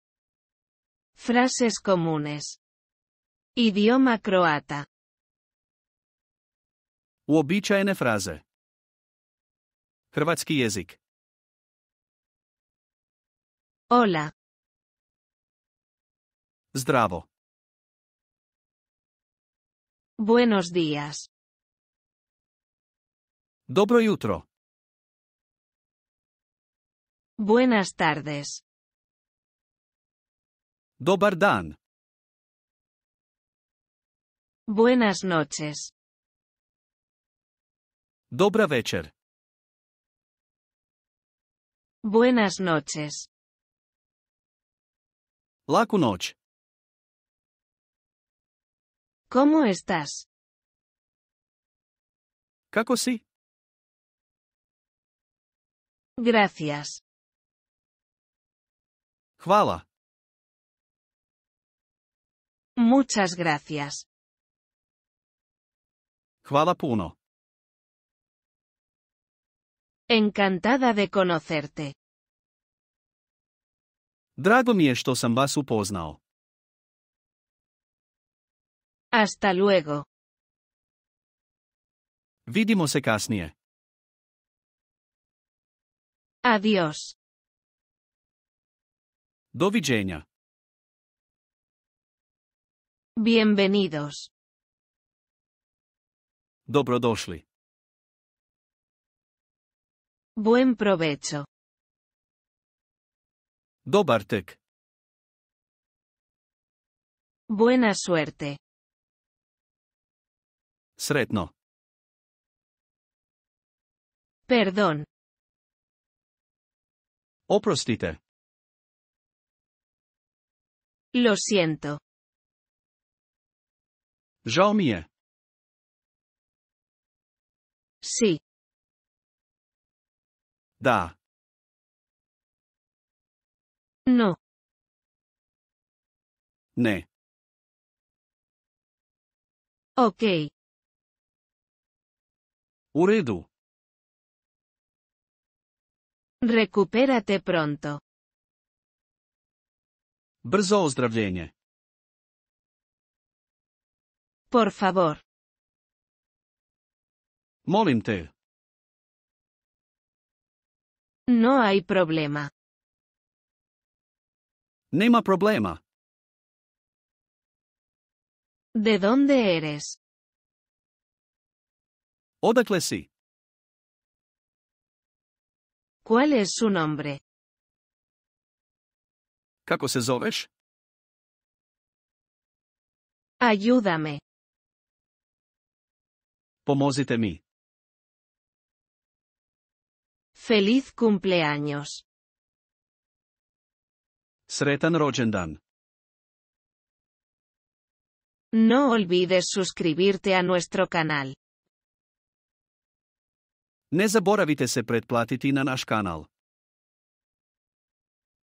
Frases comunes (Idioma croata) (es-hr)
Fone TV
es-hr-27
Uobičajene fraze (Hrvatski jezik)
Hola - Zdravo
Buenos días - Dobro jutro
Buenas tardes - Dobar dan
Buenas noches - Dobra večer
Buenas noches - Laku noć
¿Cómo estás? - Kako si?
Gracias - Hvala
Muchas gracias - Hvala puno
Encantada de conocerte - Drago mi je što sam vas upoznao
Hasta luego - Vidimo se kasnije
Adiós - Doviđenja
Bienvenidos - Dobrodošli
Buen provecho - Dobar tek
Buena suerte - Sretno
Perdón - Oprostite
Lo siento - Žao mi je
Sí - Da
No - Ne
OK - U redu
Recupérate pronto - Brzo ozdravljenje
Por favor - Molim te
No hay problema - Nema problema
¿De dónde eres? - Odakle si?
¿Cuál es su nombre? - Kako se zoveš?
Ayúdame - Pomozite mi
Feliz cumpleaños - Sretan rođendan